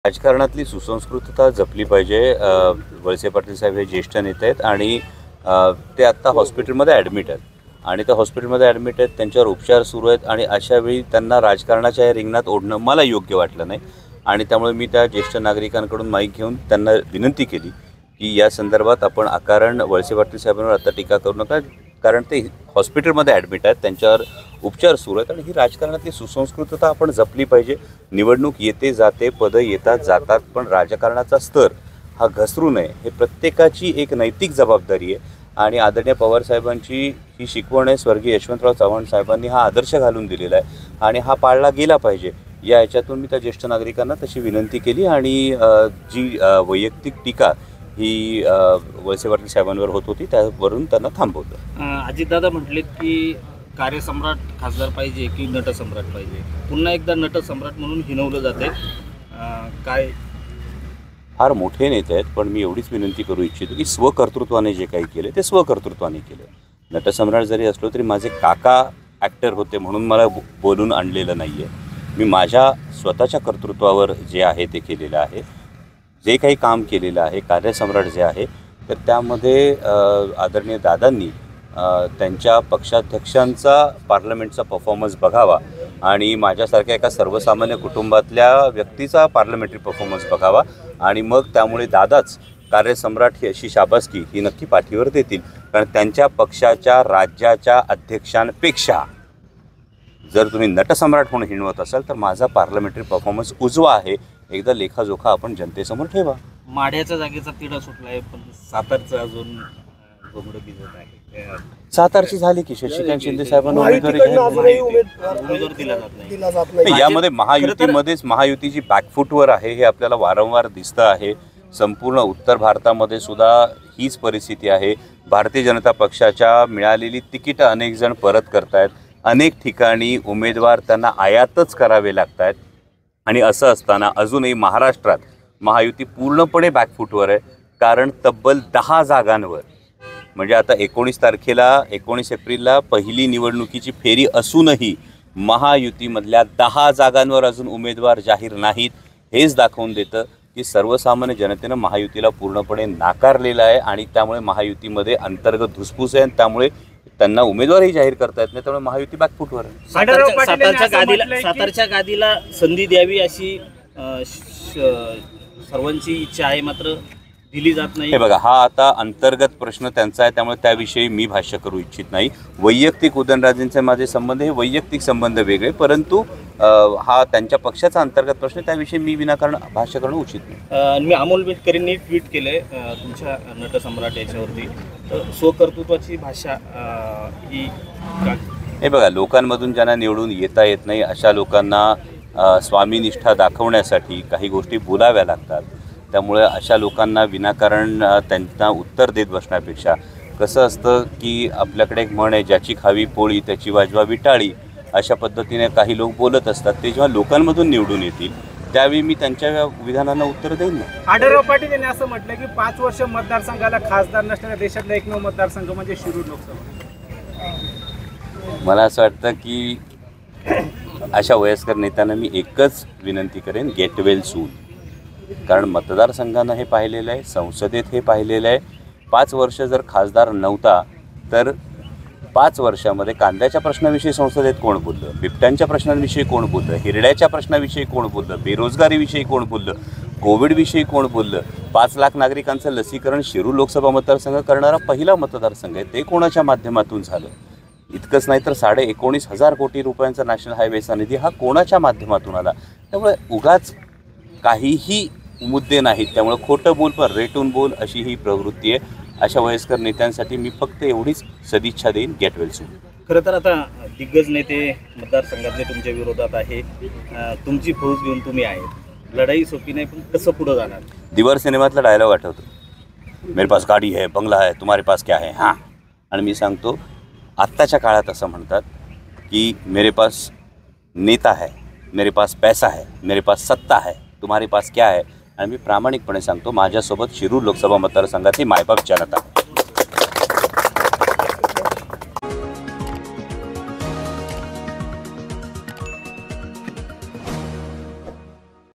Amol Kolhe | निवडणूक येते जाते, राजकारणाचा स्तर घसरू नये ही प्रत्येकाची नैतिक जबाबदारी
प्रचारात जेष्ठ नागरिकांकडून सहकारमंत्री दिलीप वळसे पाटील यांच्यावर टीका होत असताना, शरद पवार गटाचे शिरुरचे उमेदवार डॉ. अमोल कोल्हे यांनी जेष्ठ नागरिकाच्या हातातला माईक काढून घेतल्याचा प्रकार इंद्रायणीनगर भोसरीमध्ये घडला. दिलीप वळसे पाटील रुग्णालयात उपचार घेत असताना त्यांना या राजकारणाच्या रिंगणात ओढणं चुकीचं असल्याचं सांगत राजकारणातील सुसंस्कृतता प्रत्येकाने जपली पाहिजे हीच यामागील भावना होते, डॉ. कोल्हे म्हणाले. निवडणूक येते जाते, पद येतात जातात पण राजकारणाचा स्तर घसरू नये ही प्रत्येकाची नैतिक जबाबदारी आहे.

ऑन अजित पवार नटसम्राट टीका

अजित पवार यांनी हडपसरच्या मेळाव्यात, तुम्हाला कार्यासम्राट की नटसम्राट पाहिजे असं विधान करत डॉ.कोल्हेंना हिणवले, यालाच प्रतिउत्तर देताना, अजित पवार फार मोठे नेते आहेत... मी फक्त एवढचं म्हणेल, मी जे काही केलं ते स्वकर्तुत्वाने केलं, माझे काका नटसम्राट नव्हते. अजित पवार जर मला नटसम्राट म्हणून हिणवत असतील तर त्यांनी त्यांच्या पक्षाच्या प्रदेशाध्यक्षांचा संसदेतला परफॉर्मन्स बघावा, मग त्यांना कळेल त्यांच्या प्रदेशाध्यक्षकांच्या पेक्षा माझा परफॉर्मन्स नक्कीच उजवा आहे.

ऑन महायुती जागा वाटप

महायुतीला अजूनही उमेदवार आयात करावे लागतात, उत्तर भारतात हीच परिस्थिती आहे. भाजपची मिळालेली तिकीट ही परत करत आहेत. महायुती अजूनही बॅकफूटवर आहे. महायुतीत अंतर्गत धुसपुस आहे.

ऑन मिटकरी
लोकांमधून ज्यांना निवडून येता येत नाही अशांना स्वामीनिष्ठा दाखवण्यासाठी काही गोष्टी बोलाव्या लागत. ज्याची खावी पोळी, त्याची वाजवाजवी टाळी अशी आपल्याकडे म्हण आहे.

ऑन आढळराव
बेसिक मुद्दे नसल्याने खोटं बोल पण रेटून बोल अस म्हणत डॉ. कोल्हे यांनी शिवाजीराव आढळराव पाटील यांना पुन्हा एकदा गेट वेल सून अस म्हणत सदिच्छा दिल्यात

ऑन लोकसभा निवडणूक

मेरे पास गाडी हे, बंगला हे, पैसा हे तुम्हारे पास क्या है, हा...आताच्या काळात असं म्हणतात मेरे पास नेता हे, मेरे पास पैसा हे, सत्ता हे तुम्हारे पास क्या हे तर मी प्रामाणिकपणे सांगतो मेरे पास शिरुर मतदारसंघातली मायबाप जनता आहे